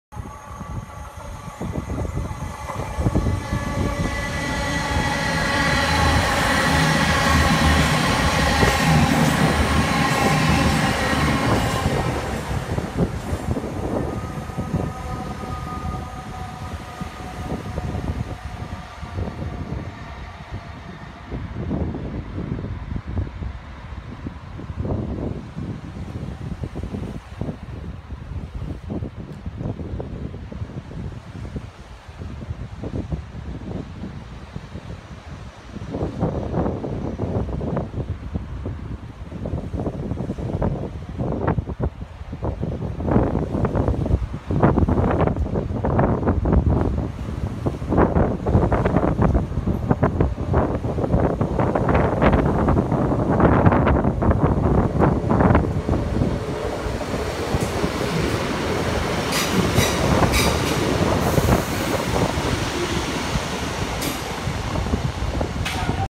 Class 220 & 150 at Dawlish.
Cross Country Class 220 and GWR Class 150/1 at Dawlish. 20/9/18